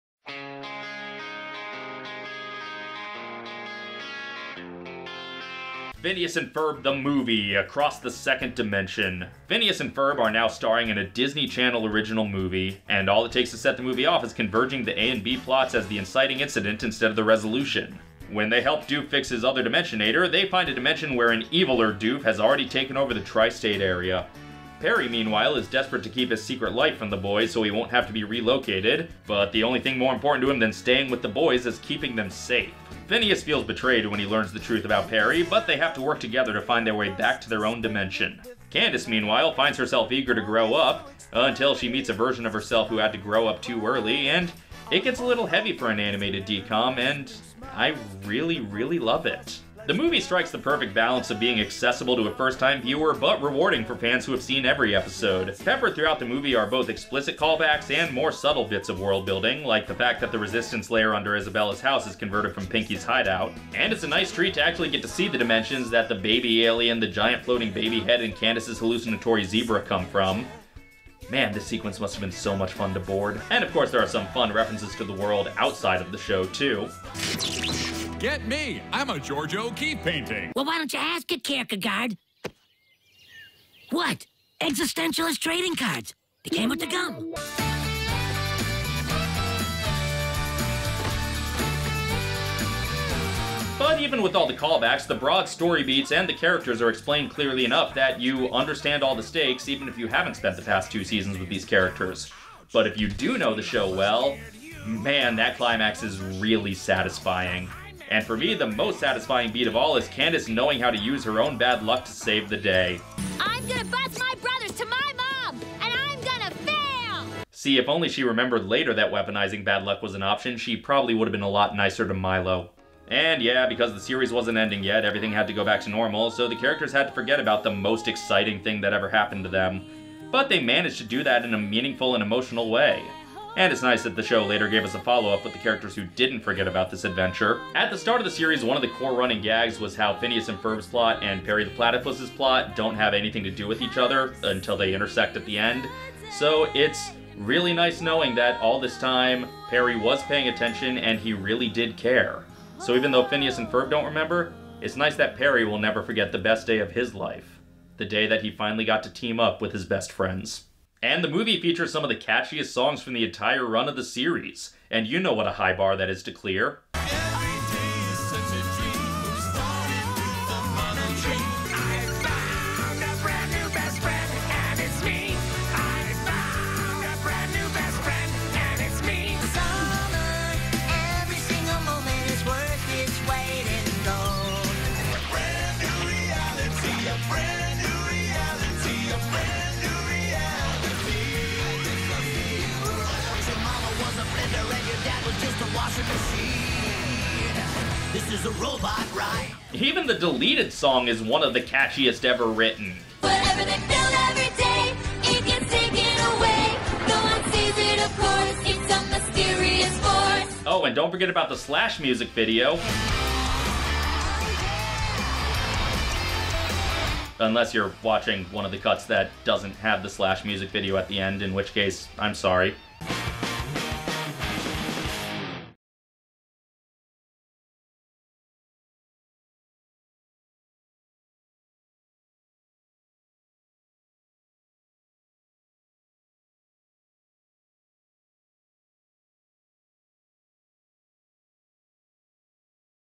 Revisiting the first PHINEAS AND FERB Movie - The Best of the D-List
With a new movie on the way, let's revisit Across the 2nd Dimension!